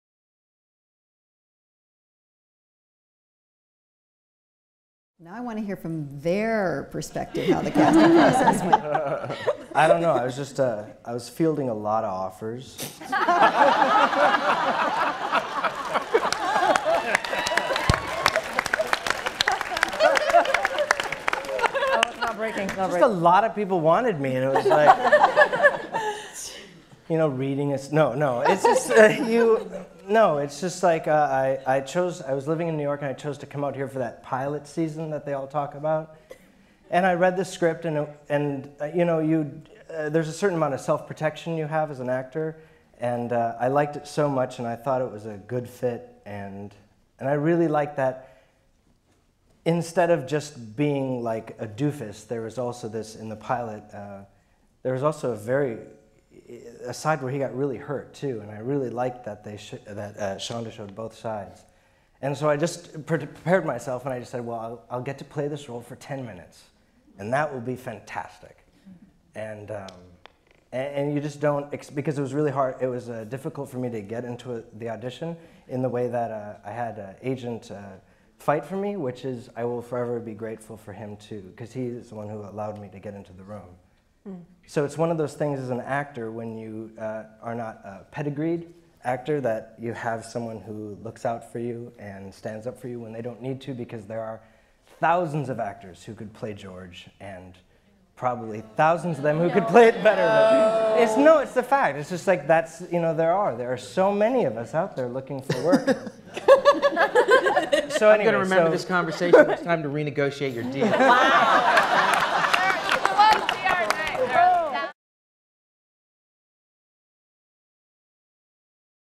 Grey's Anatomy - T.R. Knight On Auditioning
Actor T.R. Knight ("Dr. George O'Malley") tells how he got to audition for the show.